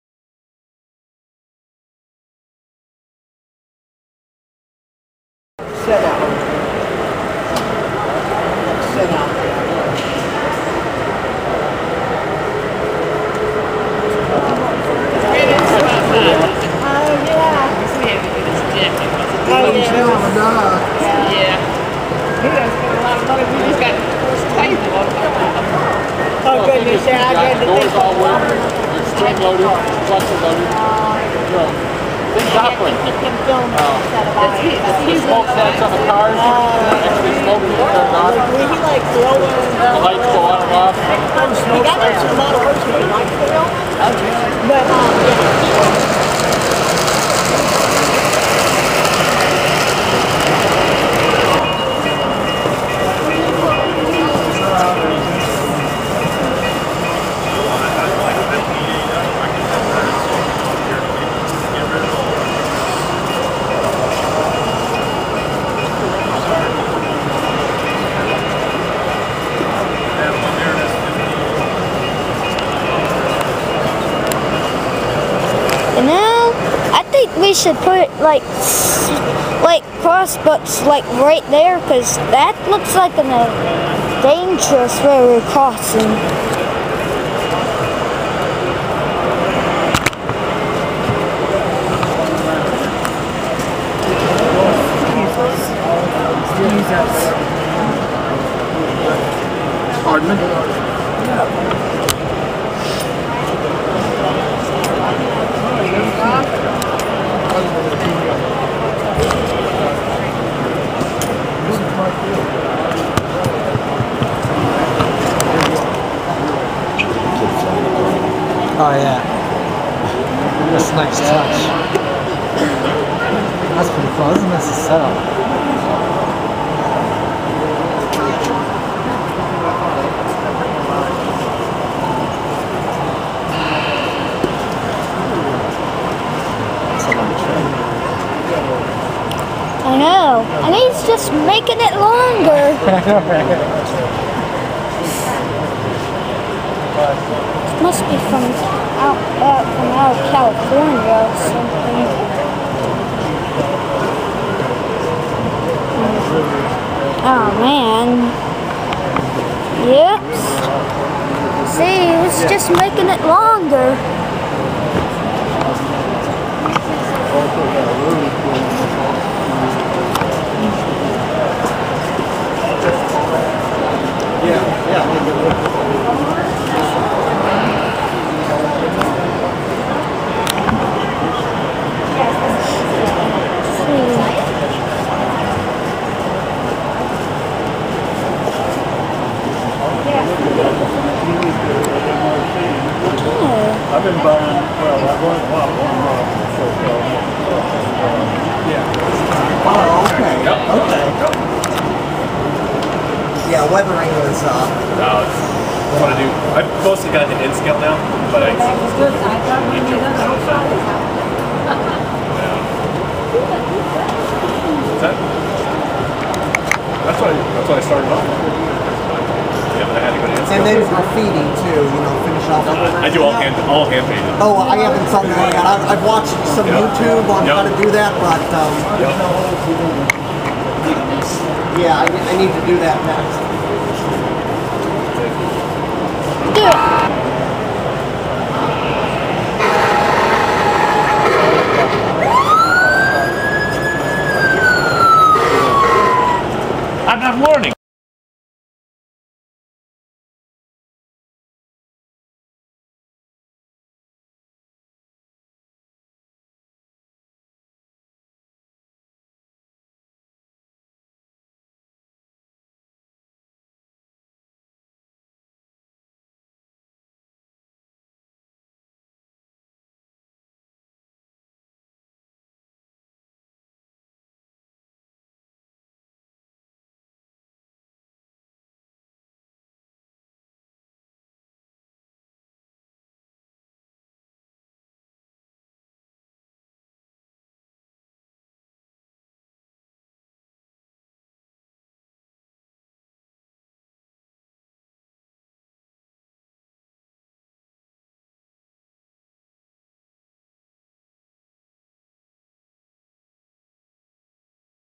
AASU train show 2013
These are videos and pictures I filmed at the anual  AASU train show. The scales are Ho, G, and O scales.